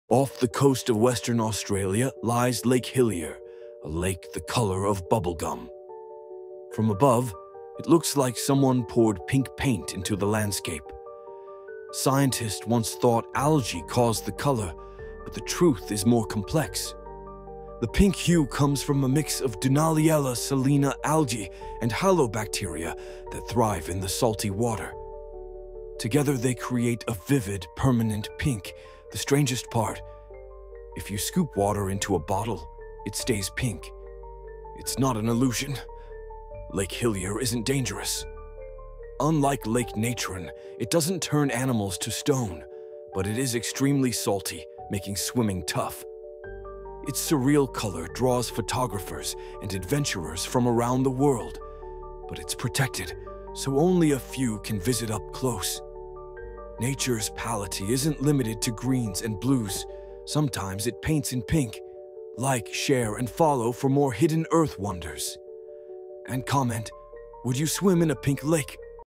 Scientists Can't Explain This Pink Lake!
Lake Hillier in Australia is naturally bubblegum pink, caused by algae and bacteria in salty water. Its color never fades.

lake hillier facts, pink lake australia, weird lakes earth, dunaliella algae pink water, hidden earth truths, science shorts